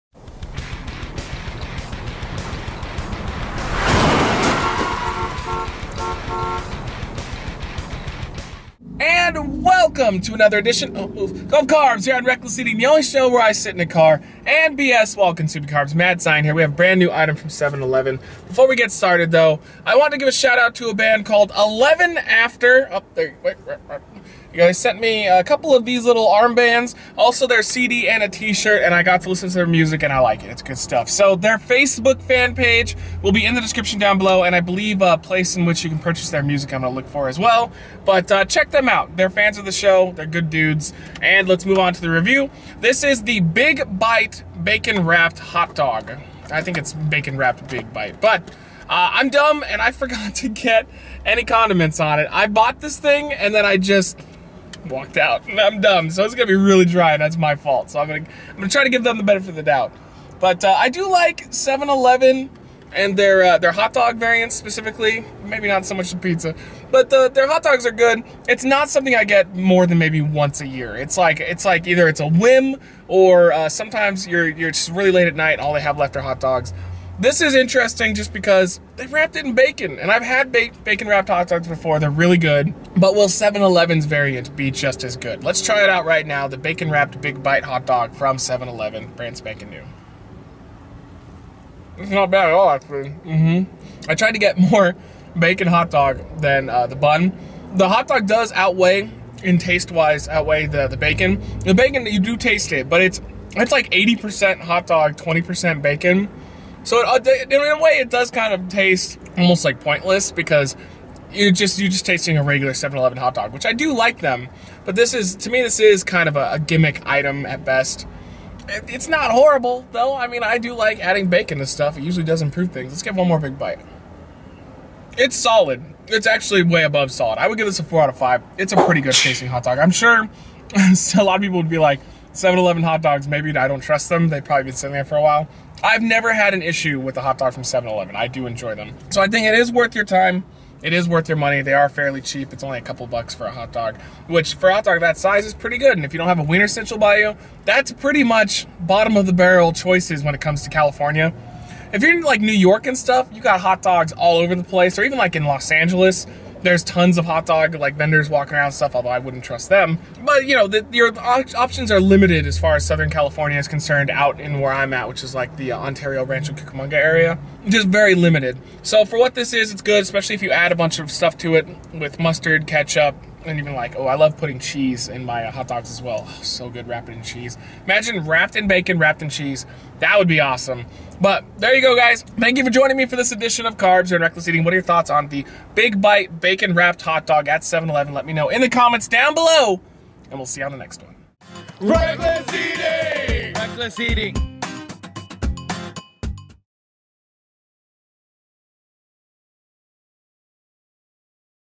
CarBS - 7-Eleven Bacon Wrapped Big Bite Hot Dog
Welcome to CarBS! On this show Matt Zion will be reviewing food/drinks in his car to get them out to the fans asap! Today Zion reviews 7-Eleven Bacon Wrapped Big Bite Hot Dog.